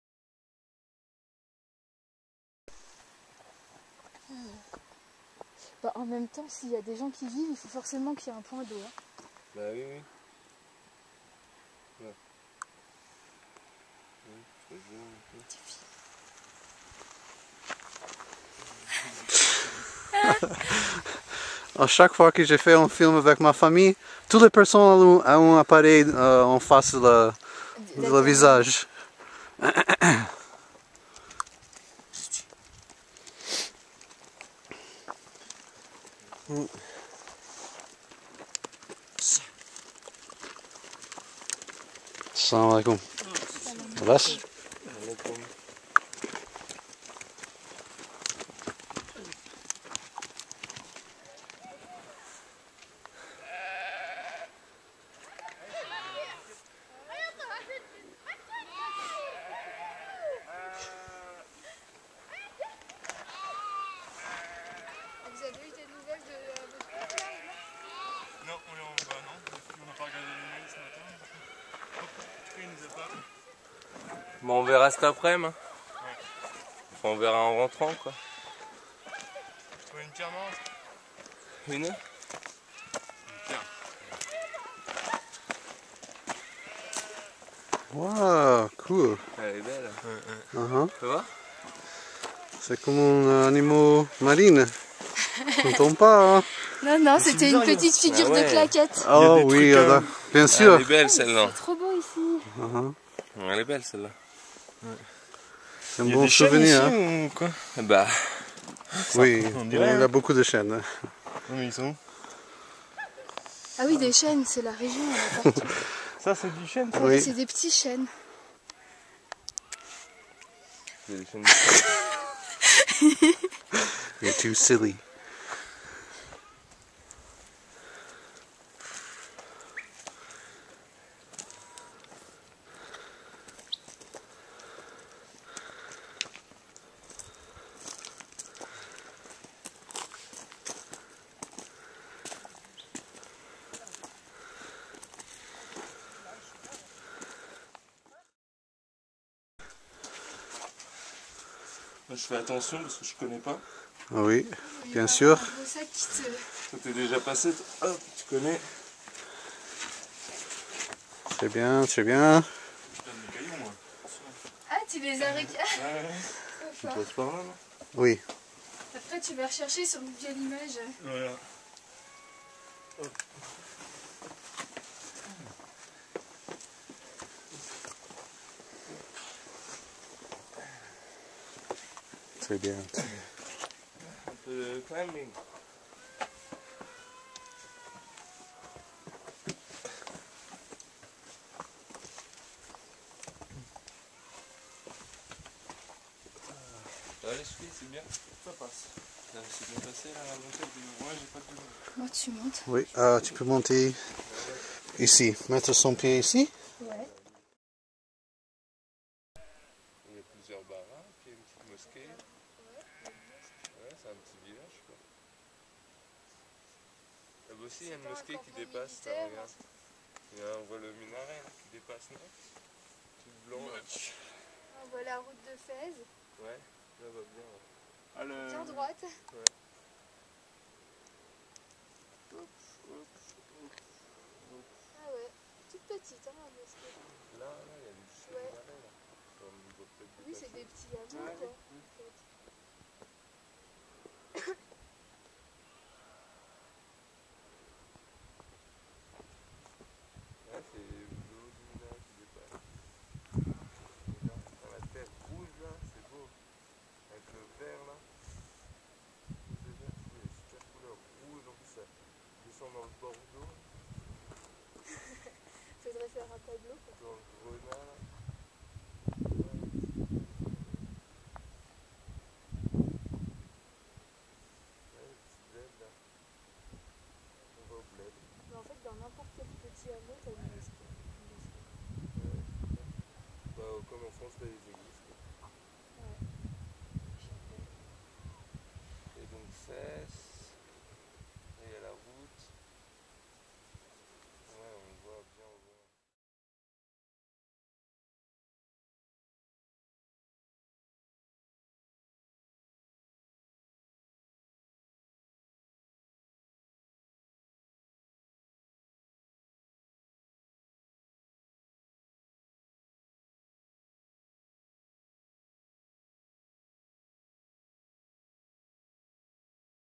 Immouzer with Camille Immouzer Kandar Middle Atlas of Morocco
MY 7 BOOKS ON AMAZON IN 13 COUNTRIES:
3 books are about my world travels, 3 children's books, and 1 self-help book. Available in Kindle and paperback editions.

Camille and I went for a walk in the Immouzer mountains of Middle Atlas Morocco.